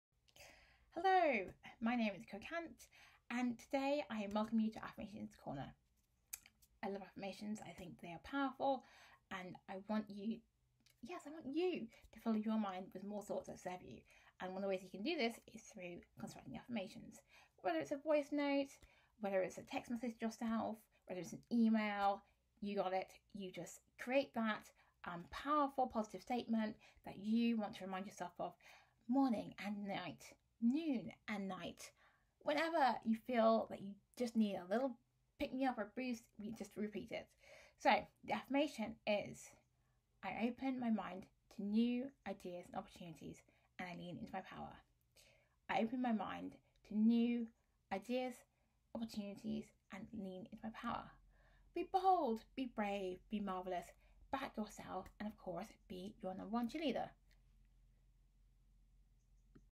Be Your No. 1 Cheerleader: Affirmations Corner 1
Hello,

This video is the first of the Affirmations Corner Series. The affirmation is:

I open my mind to new ideas, opportunities and I commit myself to actively leaning into my power.

Warm Regards,

Ekua 💛